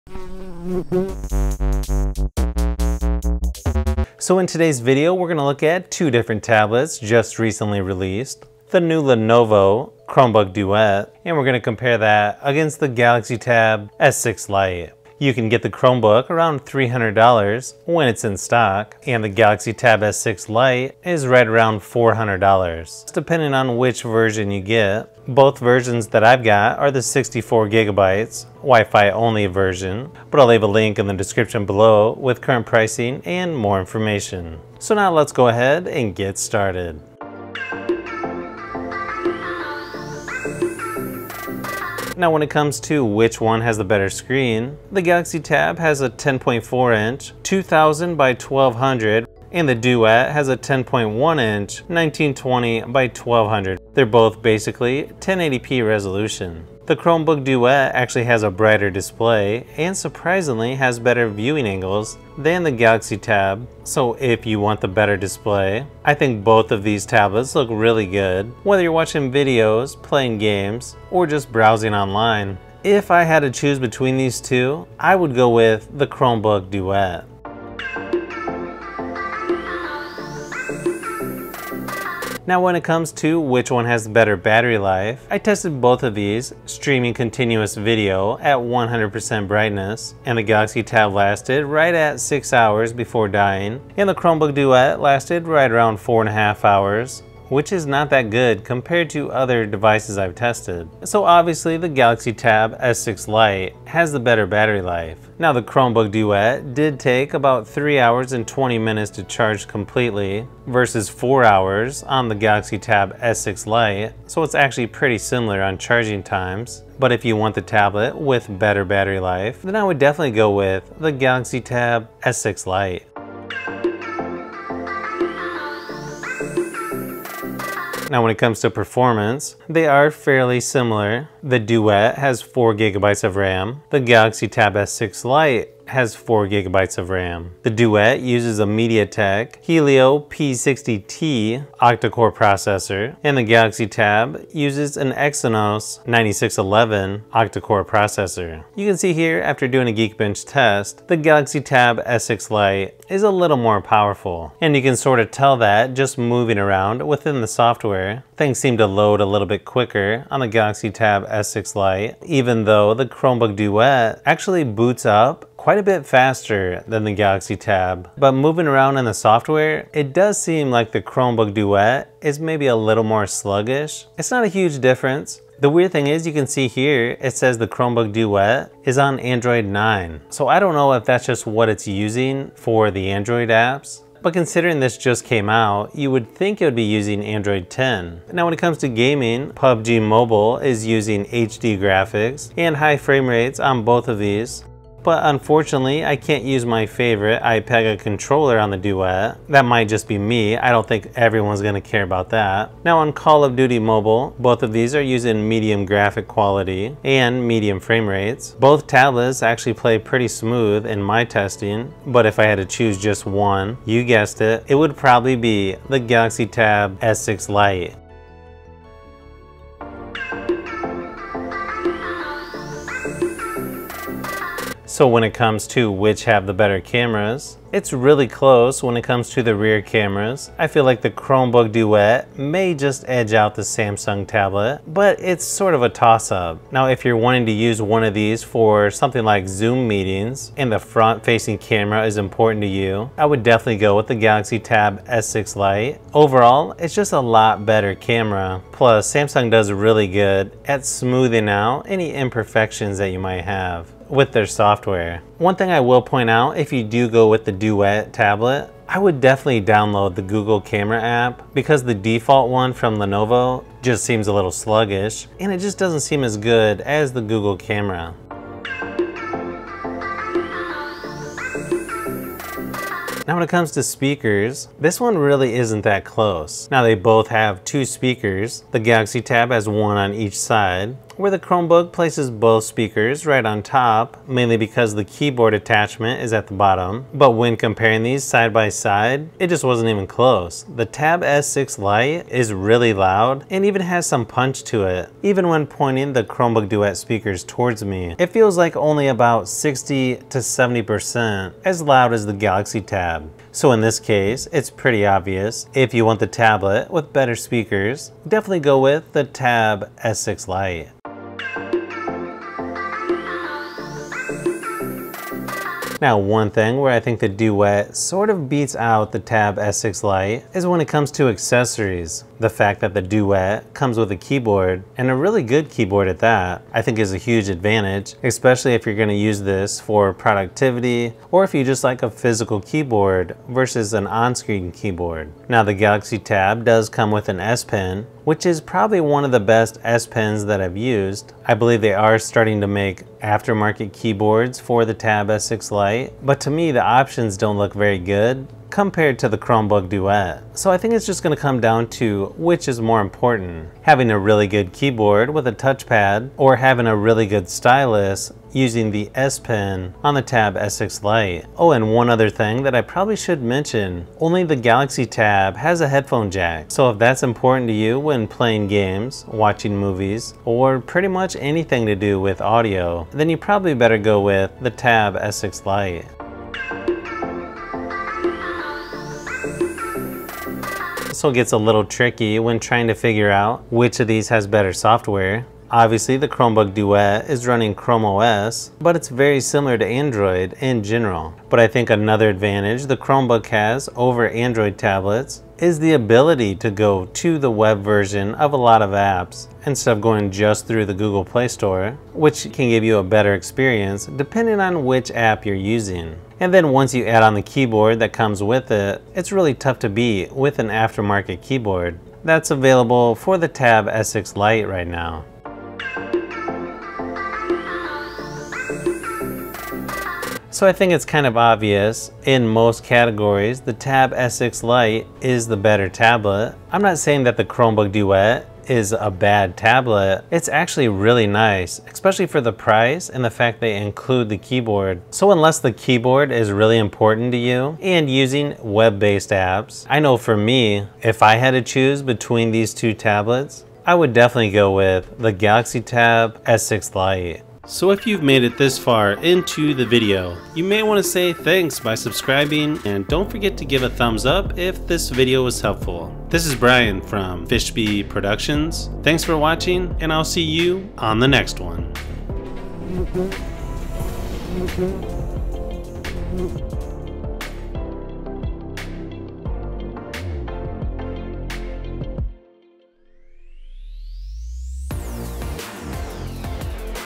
Lenovo Chromebook Duet vs. Galaxy Tab S6 Lite - Which is Better?
Lenovo Ideapad Chromebook Duet vs. Samsung Galaxy Tab S6 Lite - Which is Better, Chromebook or Android?

Chromebook Duet on Amazon

Samsung Galaxy Tab S6 Lite on Amazon

Capacitive Stylus

Infiland Folio Business Case

Infiland Folio Multiple Angle Case

Flydigi Apex Controller

iPega 9167 Controller